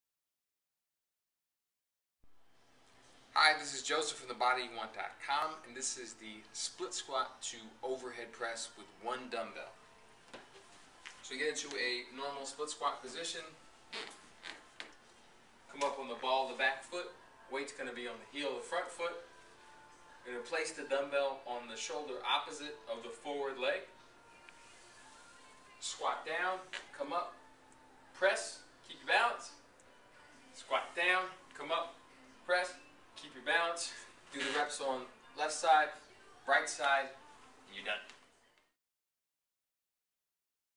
Washington DC Fitness Coach Split Squat to Overhead Press
Award winning Washington, DC Fitness coach and author Josef Brandenburg shows you how to do a split squat to overhead press.